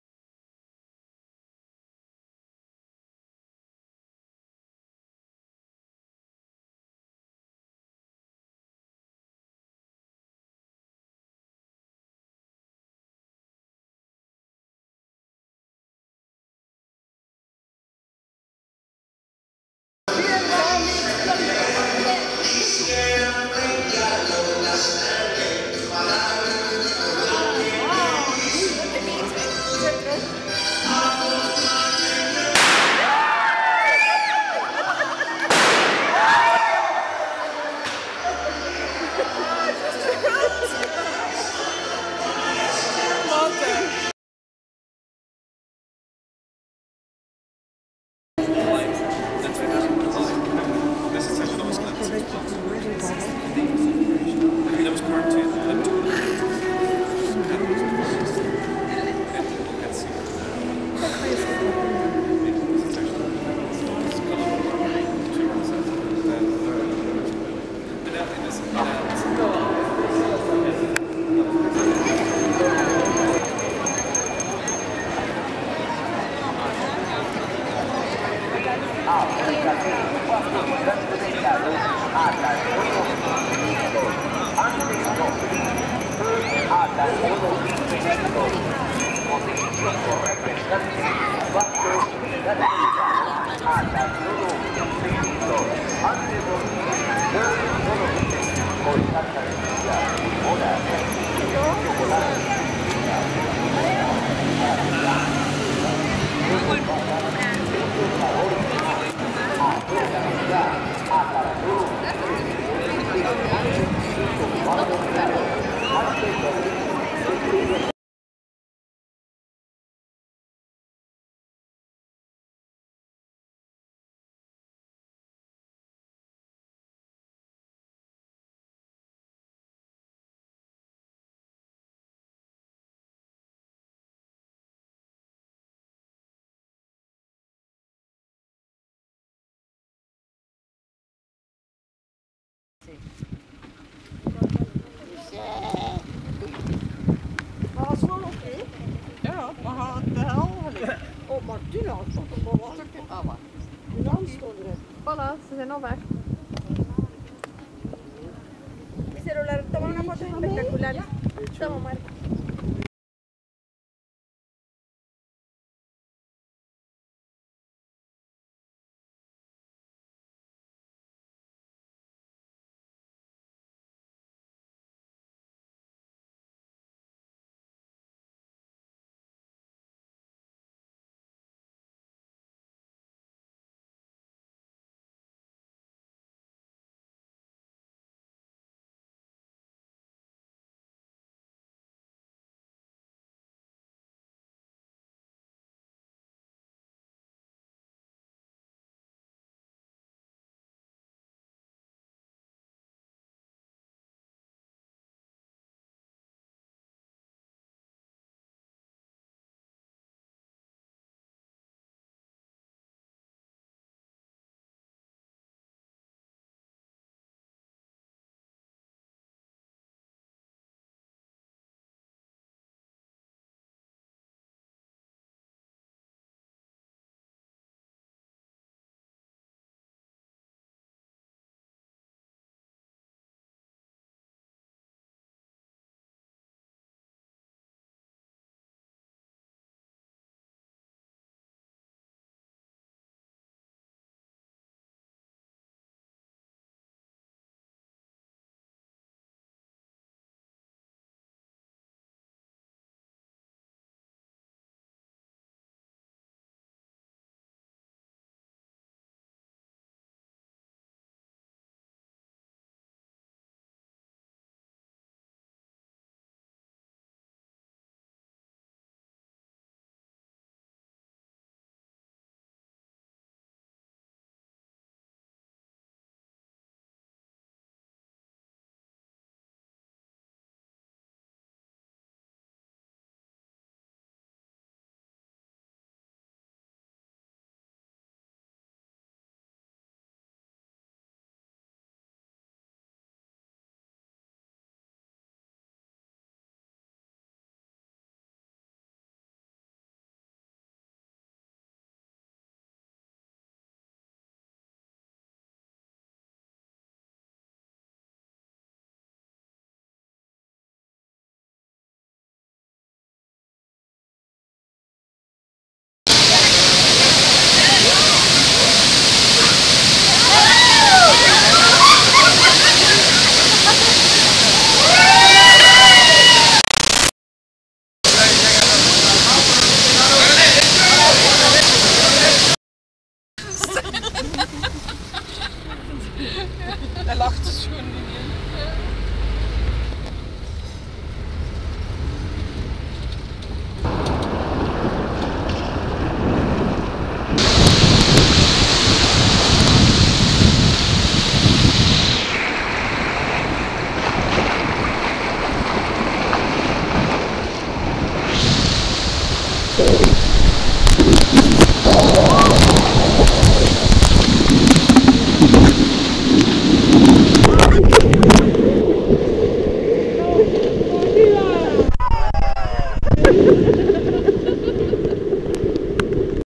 Colombia 2019 - 5 chicas deel 1
With 5 ladies in Colombia, on a self organised 3 weeks trip